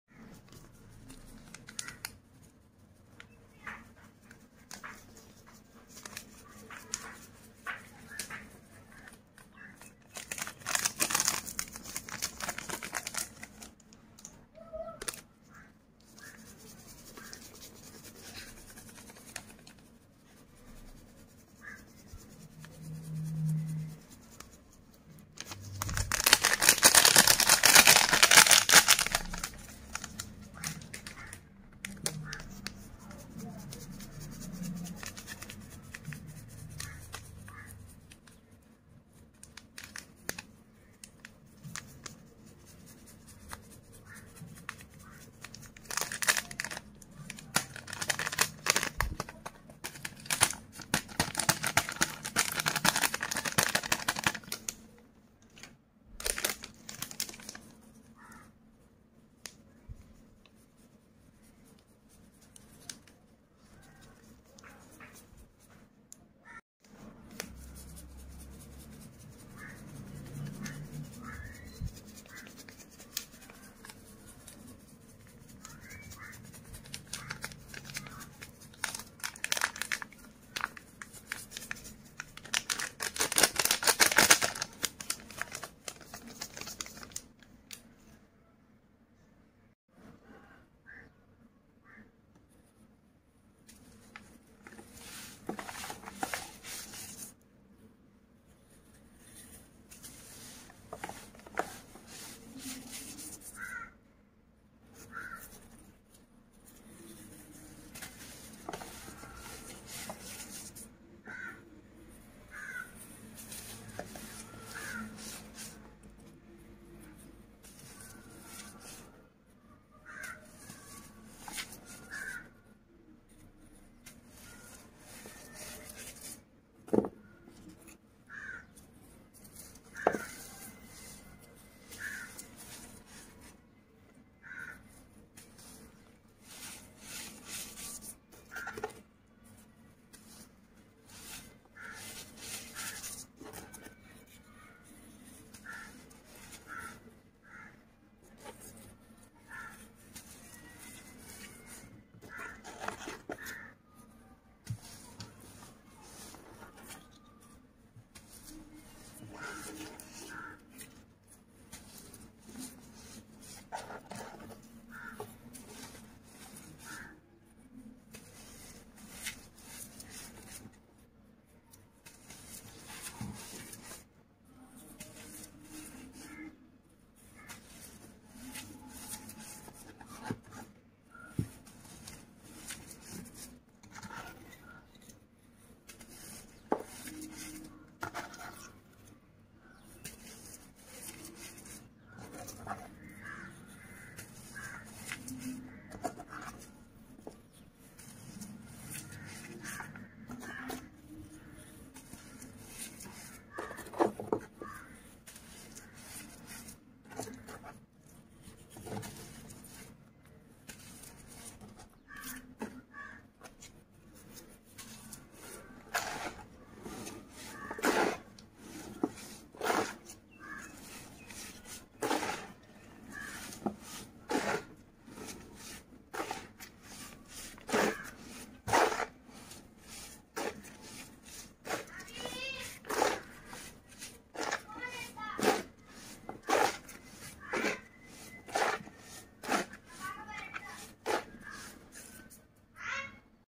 ASMR - Donut collab with my glittery queen @Messy Jessy ASMR 🖤 oddlysatisfying | read description 👇
Today's Collab with the gorgeous 🤍 @Messy Jessy ASMR
I was so happy that I'm doing Collab with you...she is so sweet and amazing person 🥰 ...

And I most loved that she call me Jenn and I call her Jess😋❤️..

I thank god for giving me a wonderful and supporting friend 🖤